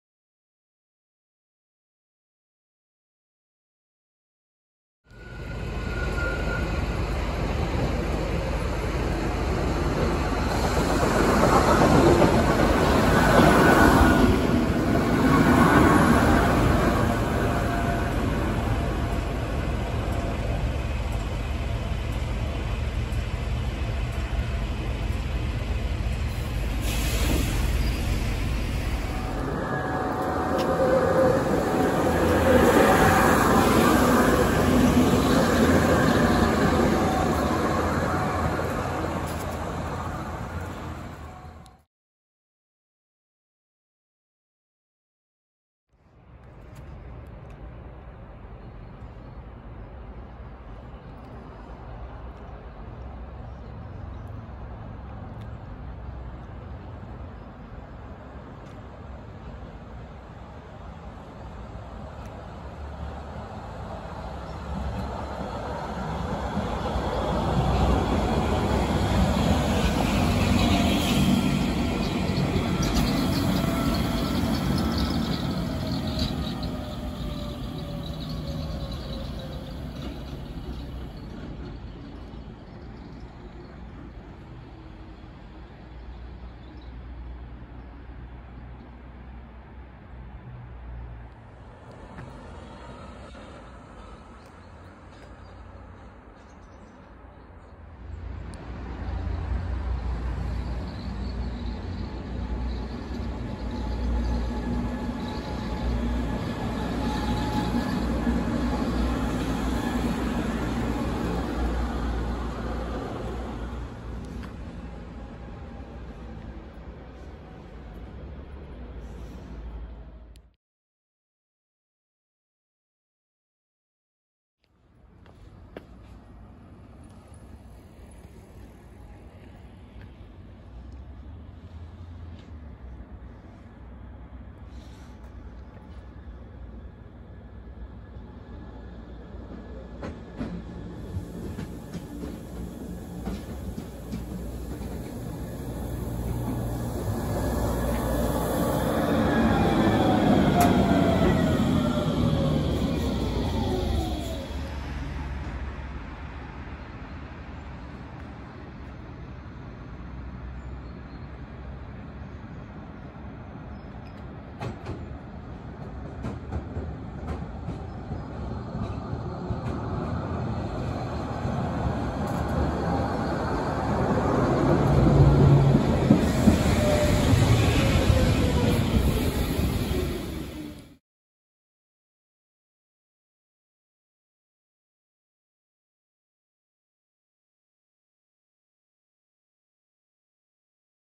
Stuttgart‘s rack and pinion tramline
Line 10 of the Stuttgart tramway network is unusual because it is a rack railway, which runs up side of the Neckar valley in the south of the city.
This video follows the line from the upper terminus at Degerloch down to the passing loop at Wielandshöhe.